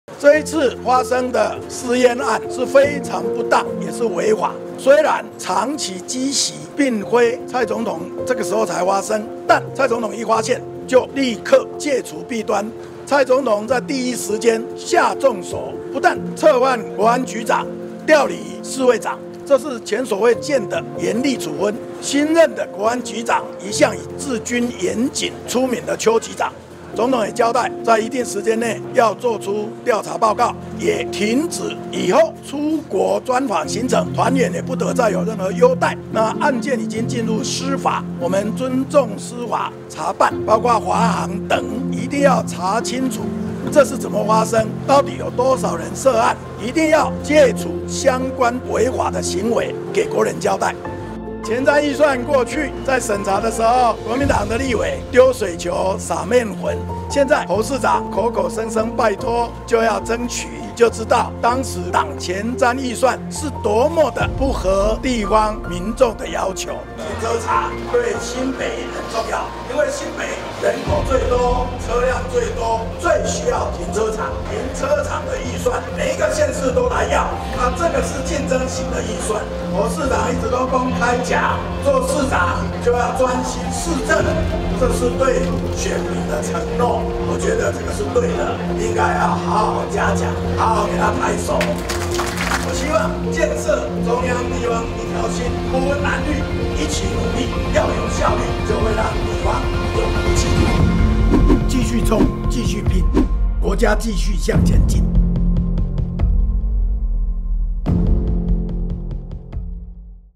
專機私菸事件 蘇貞昌這樣說 | 行政院長蘇貞昌
這一次發生的私菸案非常不當，也是違法。

蔡總統在第一時間撤換國安局長、調離侍衛長，這是前所未見的嚴厲處分。新任國安局邱局長一向以治軍嚴謹出名，總統也交代在一定時間內要做出調查報告。以後出國專訪行程，團員不得再有任何優待。

這雖然是個長年積習，但一發現就應該要立刻徹查弊端，痛改前非才對。相關的責任一定都要查清楚，唯有完全戒除這種行為，才能給鄉親交代。

今天我到了三重，參加前瞻基礎建設計畫補助的地下停車場啟用，我也大力稱讚侯市長恪守職責、努力打拚的精神，請看今天的影片。

大家有空嗎？